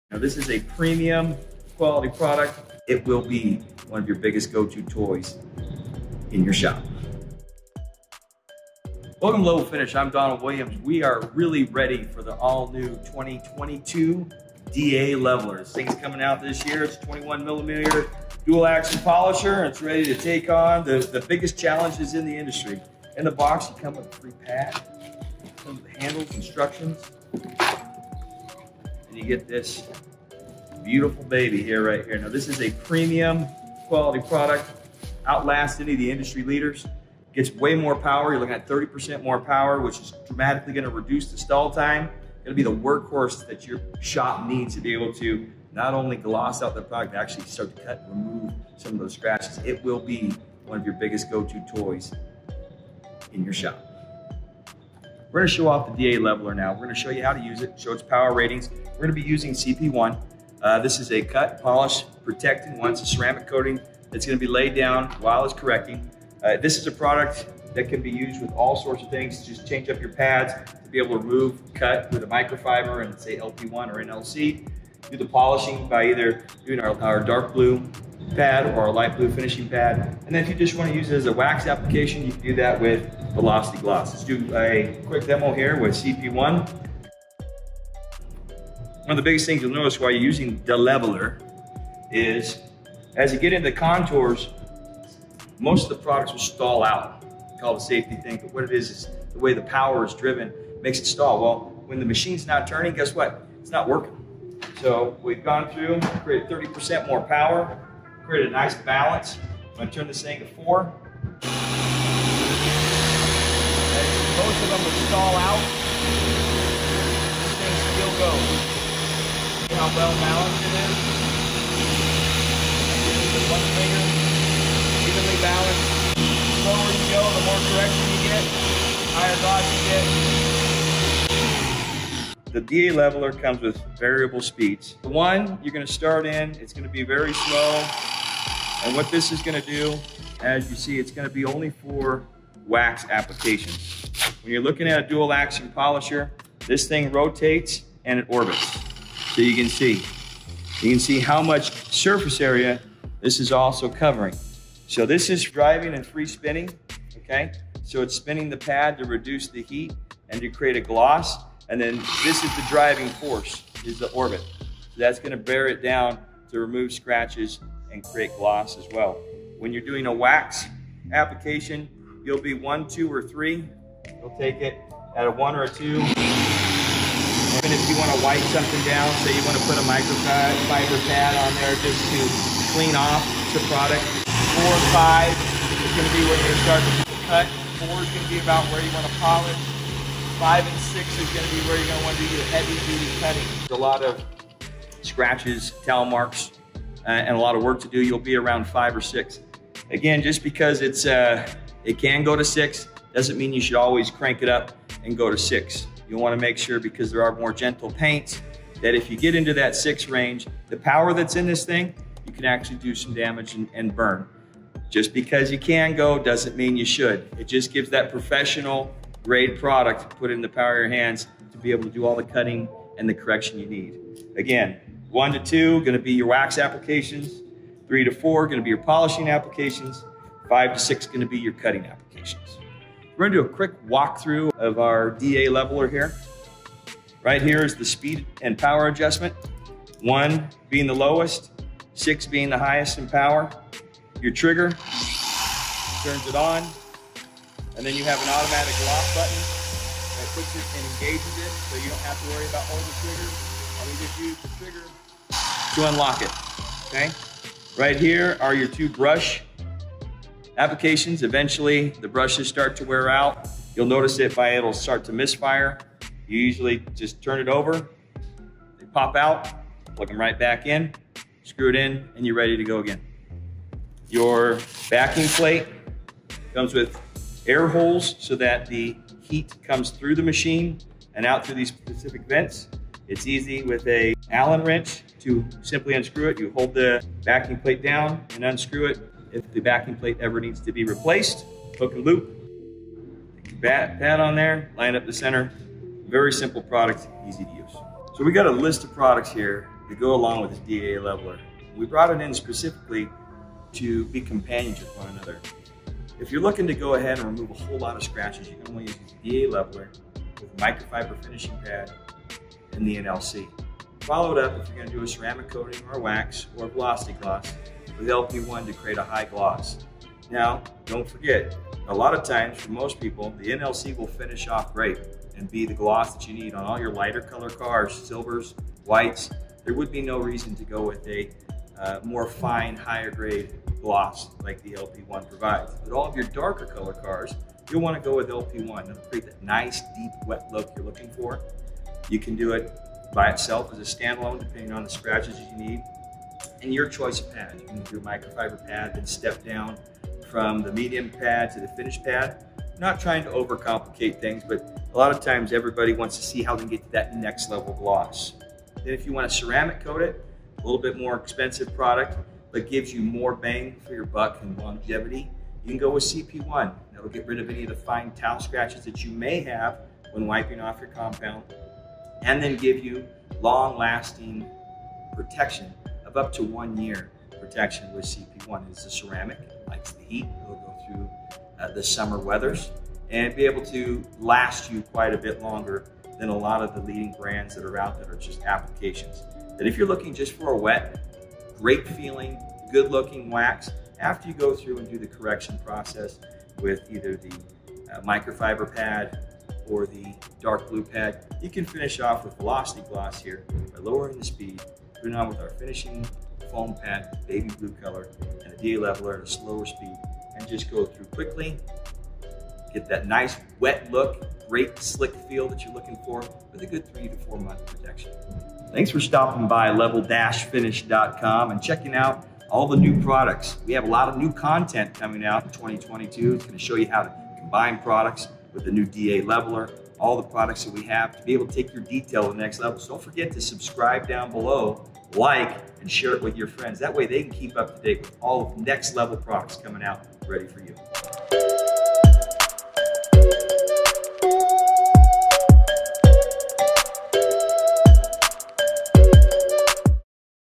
Introducing the new 2022 DA Leveler unboxing, demo & product combo walkthrough
The DA Leveler Dual Action polisher "DA Leveler HD" is a workhorse that will take your paint correction jobs to the NEXT LEVEL. The DA Leveler provides up to 30% more power to the buffing surface, increasing balance, and greatly reducing the stall rate. This premium Dual Action 21mm orbital polisher has been tested in Body Shops, Detail Centers, and OEM markets,  and proven to tackle the biggest jobs and we are confident you are going to love it. DA Leveler combines comfort, power, performance, and dependability making it the workhorse your shop needs!  Level Finish's products are designed to help our clients a quality finish with shorter cycle times and increased productivity.